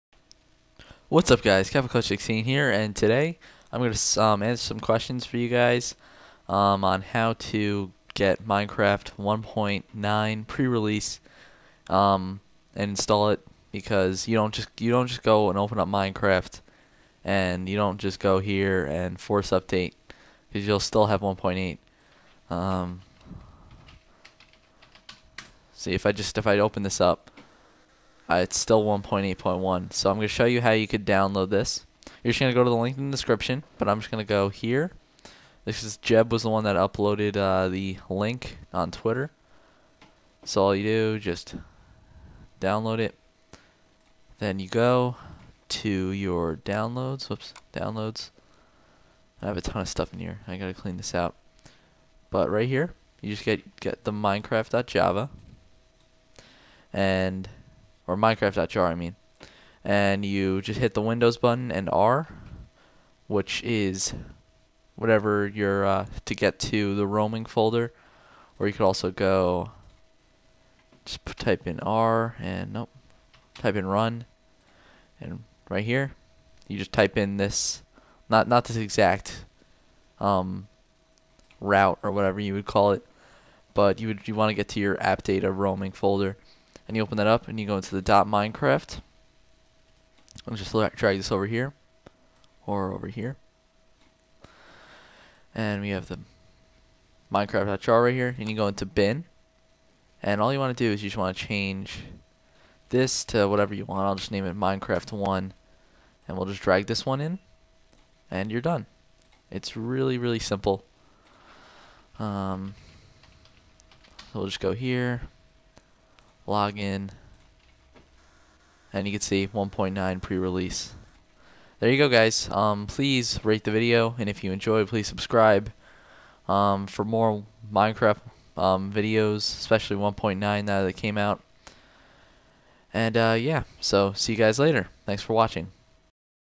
How to Download and Install Minecraft 1.9 Pre-Release
A tutorial on how to download Minecraft pre-release.